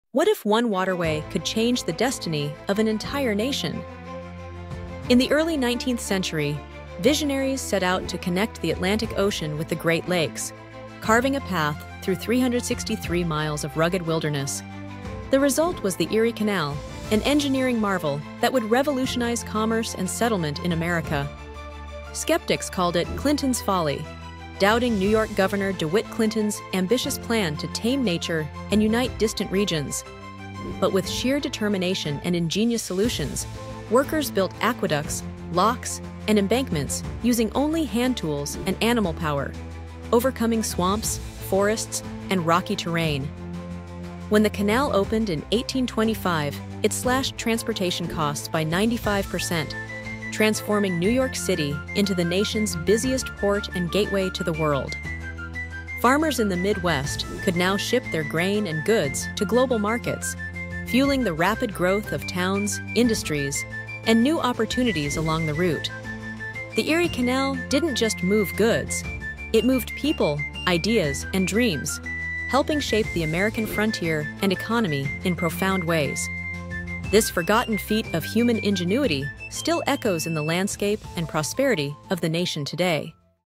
How One Canal Changed America!
Discover the untold story of the Erie Canal—an engineering feat that transformed a nation. Dive into history and see how innovation reshaped America!